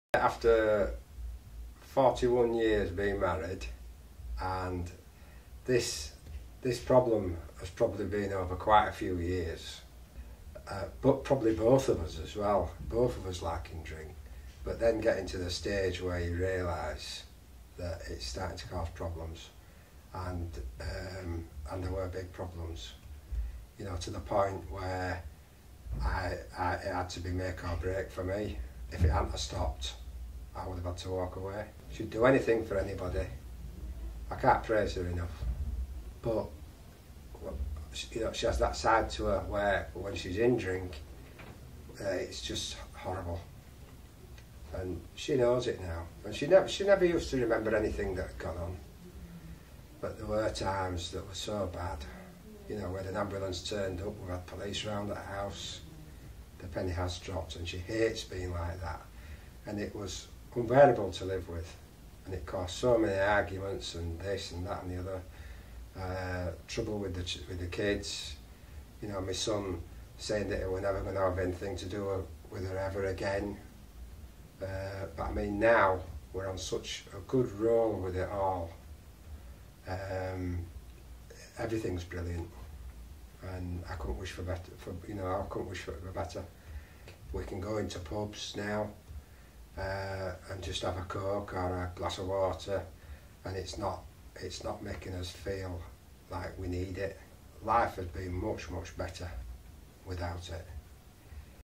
Andrew on how his wife quitting drinking has improved their life
Andrew opens up about the hard realities of loving someone with alcohol issues. Andrew talks about how much better their life is now.